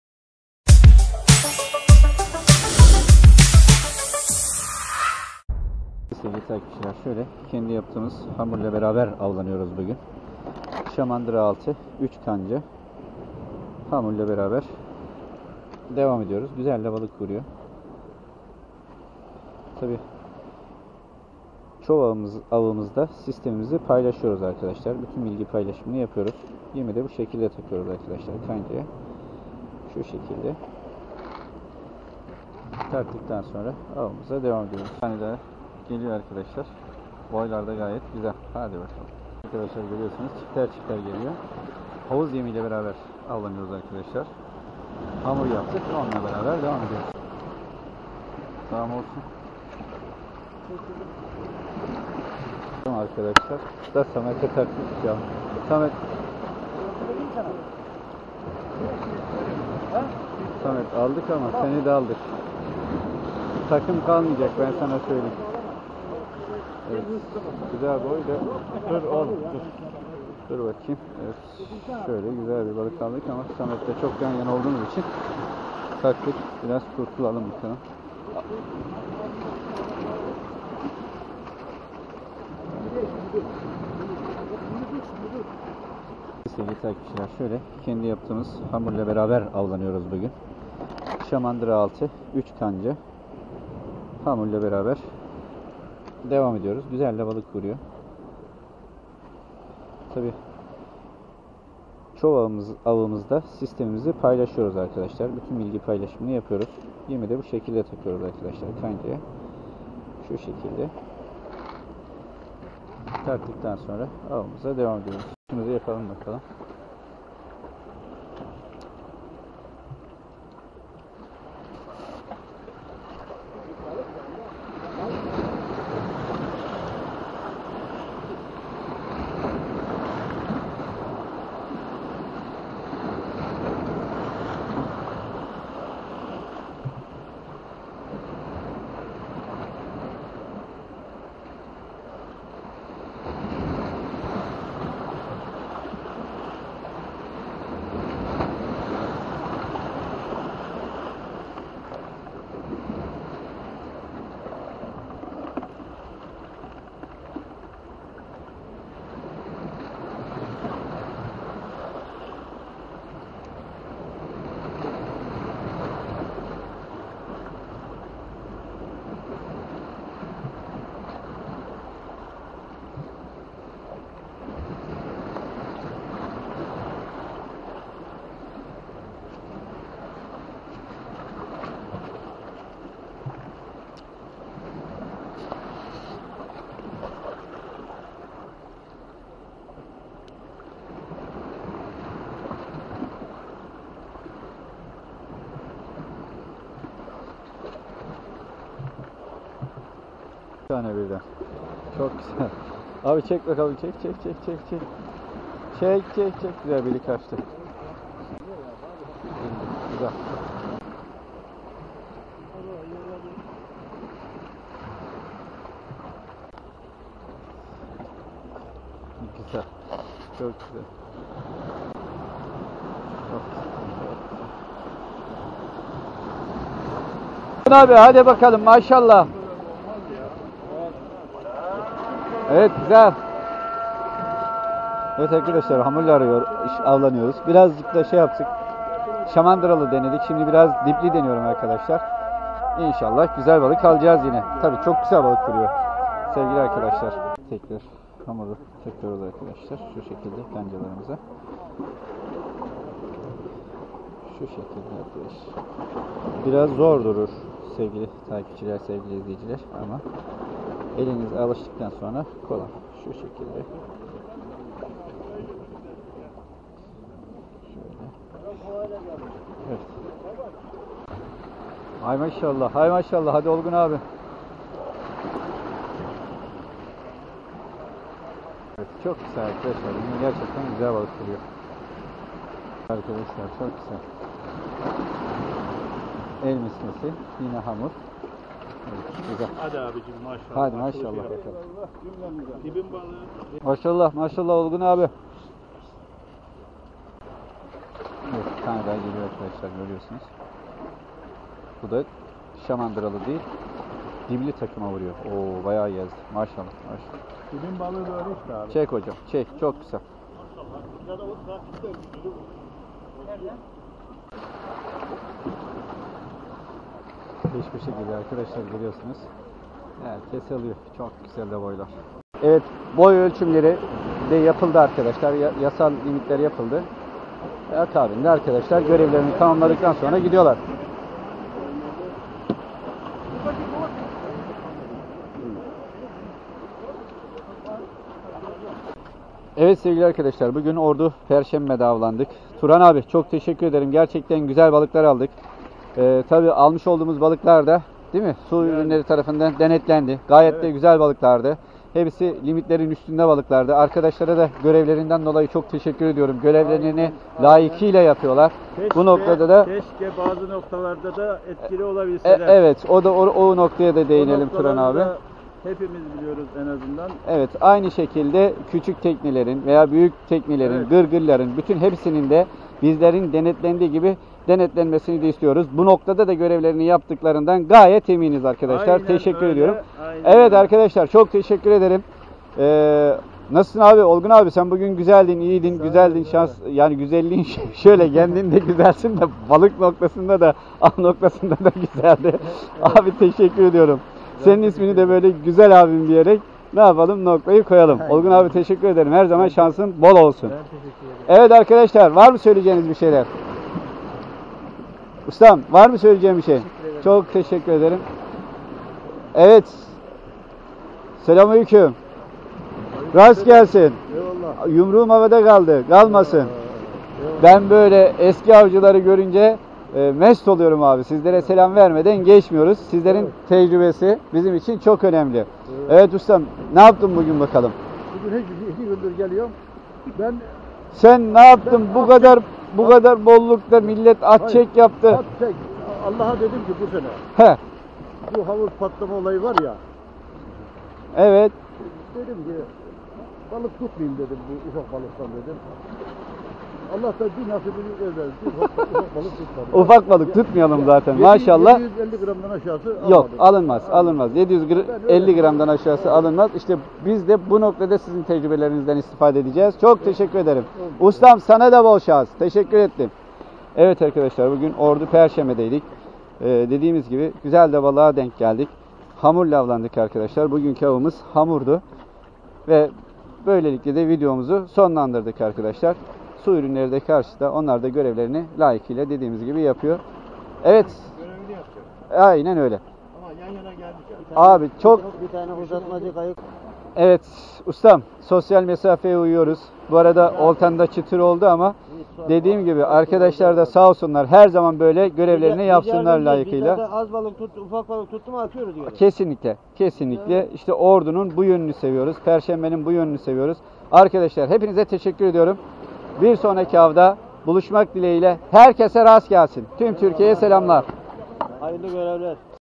ŞAMANDIRA ALTI ÜÇ KANCA, YEM HAMUR, ÇİFTLİK KAÇKINI LEVREK AVI KARADENİZİN MAVİ DERİNLİKLERİ 🇹🇷🇹🇷🇹🇷
Ayrıcalıklardan yararlanmak için bu kanala katılın:
Ordu ili Perşembe ilçesinde şamandıra altı üç kanca,Yem kendi yaptığımız hamuru tercih ederek avlandık.Bu günde Güzel ava denk geldik.Bir sonraki avda birlikte olmak temennisi ile,Bizi takipte kalın.